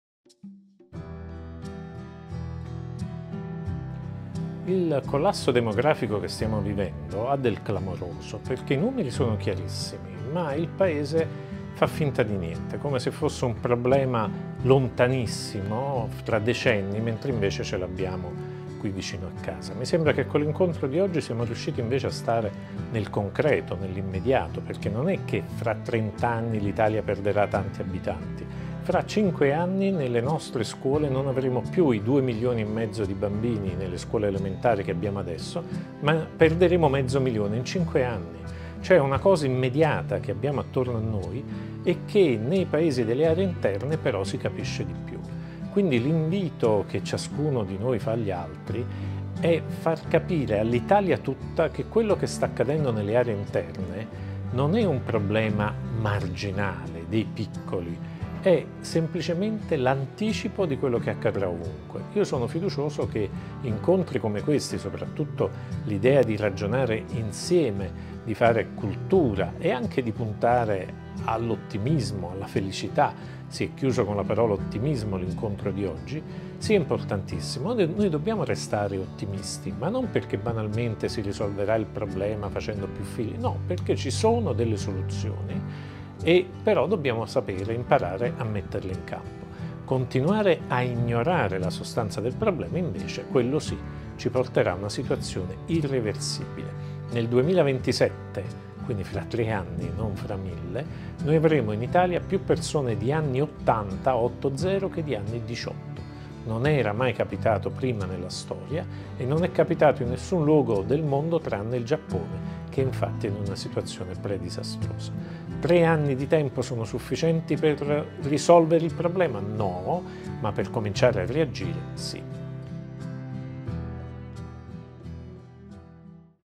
SUMMER SCHOOL 2024_INTERVISTA A MARCO ESPOSITO
🎙️ Abbiamo intervistato Marco Esposito, scrittore e giornalista, in occasione della Summer School 2024 organizzata a Pietrelcina (Benevento) dal Laboratorio per la Felicità Pubblica